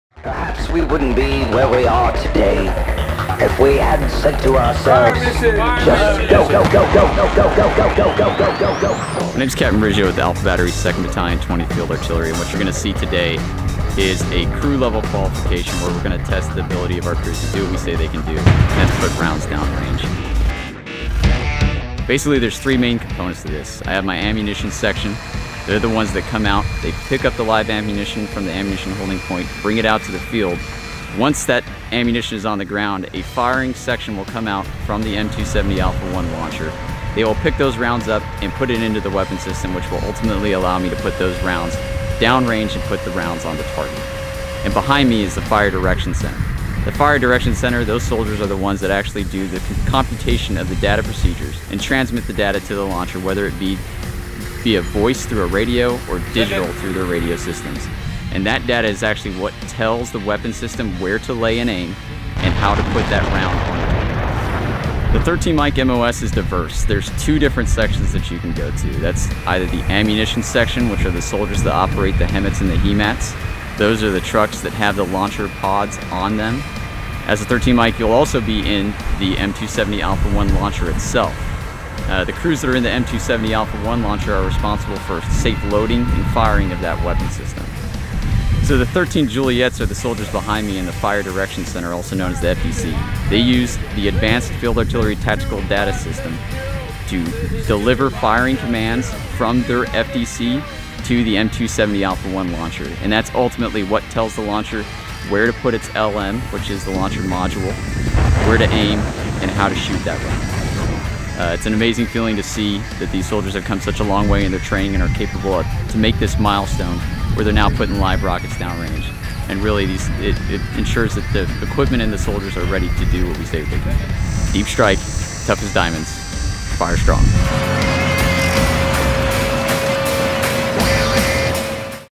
MLRS: What it takes to fire rockets
During crew-level certification training with A Battery, 2nd Battalion, 20th Field Artillery, Capt. Jordan Riggio, A/2-20th FA Commander, explains what it takes to fire a rocket downrange. Much more complex than pushing a button, firing a rocket from the Multiple Launch Rocket System (MLRS) requires three teams comprised of soldiers from two separate career fields.